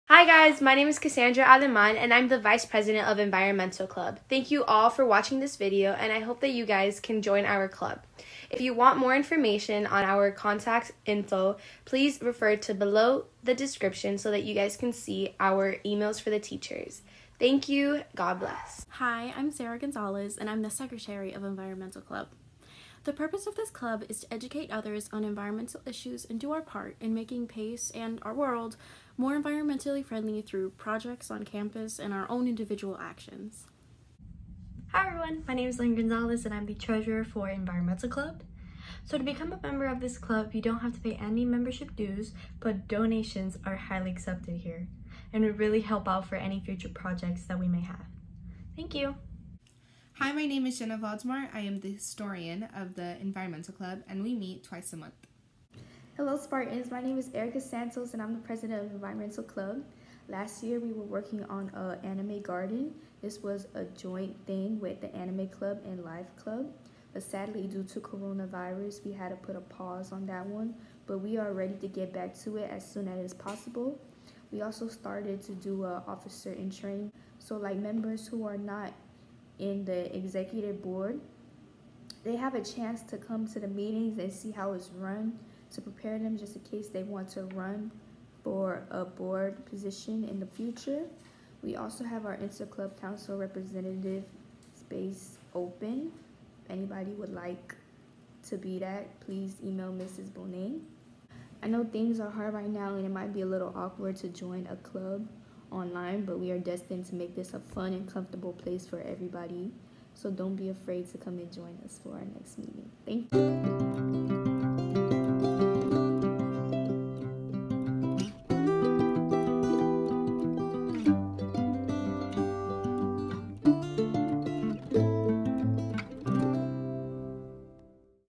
Environmental Club Video 2020-2021
Environmental Club Officers for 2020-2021 introduce themselves and talk about what it is to be part of the Environmental Club at Msgr Pace High School.